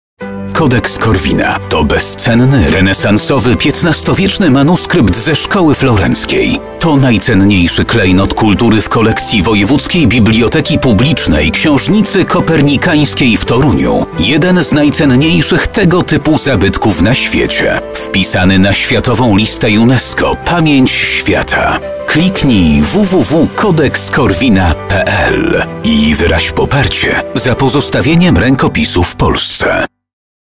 Petycja w sprawie kodeksu Korwina
WYRAŹ SWOJE POPARCIE I PODPISZ DEKLARACJĘ

poparcia dla zachowania w narodowym dziedzictwie kultury Rzeczypospolitej Polskiej „Katalogu Korwina” - bezcennego, renesansowego manuskryptu szkoły florenckiej, wpisanego na Światową Listę Programu UNESCO "Pamięć Świata" - najcenniejszego zabytku kultury w kolekcji Wojewódzkiej Biblioteki Publicznej - Książnicy Kopernikańskiej w Toruniu

O manuskrypcie

Kodeks jest jednym z najcenniejszych rękopisów XV-wiecznych w Polsce. Znajduje się w Toruniu od XVI wieku i stanowi część zabytkowego księgozbioru Książnicy Kopernikańskiej. Z uwagi na okazaną mu troskę oraz ponad 400-letni okres przechowywania dzieła w Toruniu jest ono nierozerwalnie powiązane ze znajdującym się na Liście Światowego Dziedzictwa Kulturowego i Naturalnego UNESCO miastem oraz jego historią.